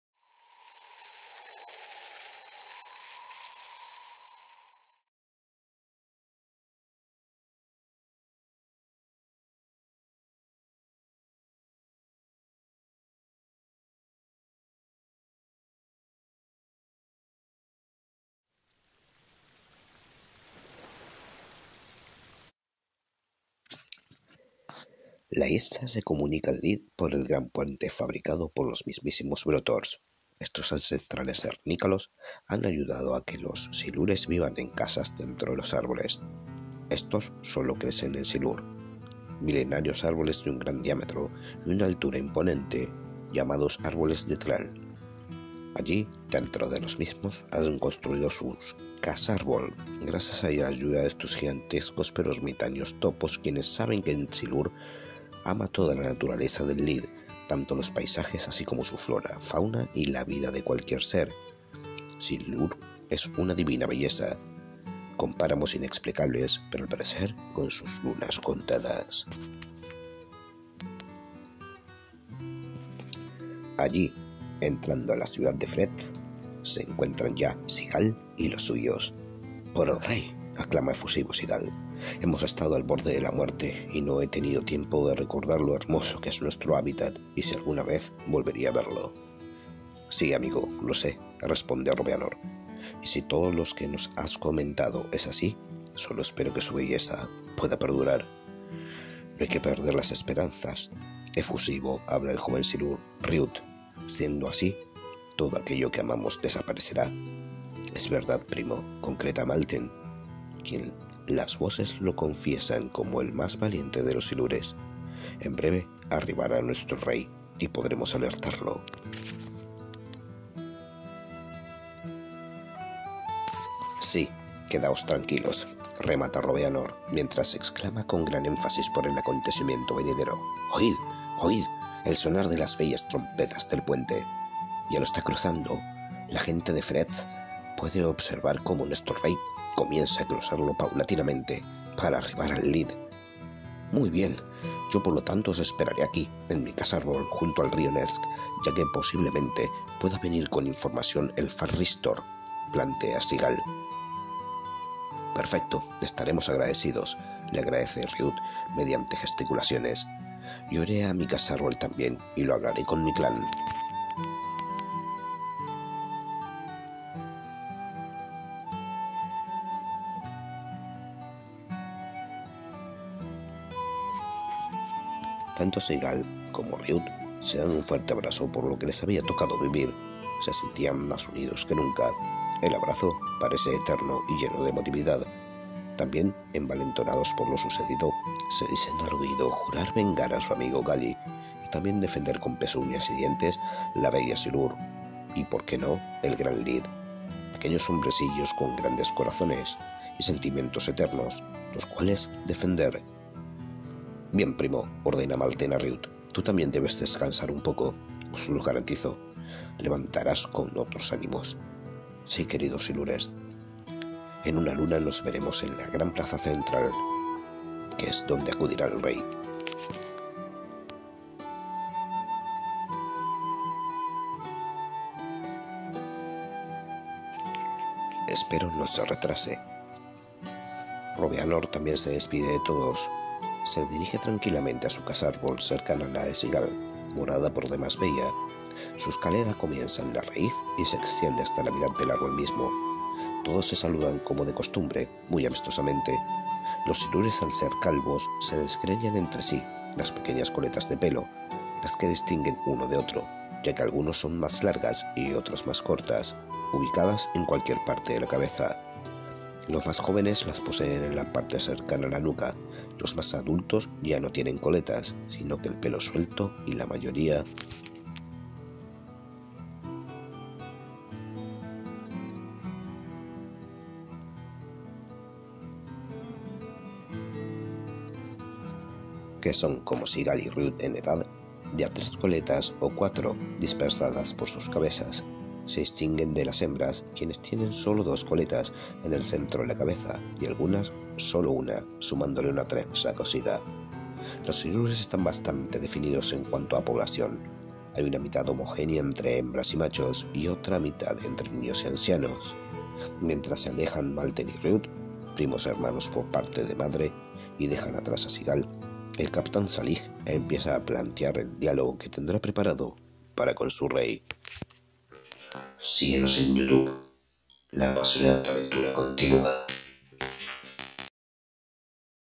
♟Fantasía Medieval Audiolibro Audiobooks sagafantasticaellid Audiolibro 06x05
Adéntrate en la región  de silur, y conoce sus costumbres, mientras los Silures esperan noticias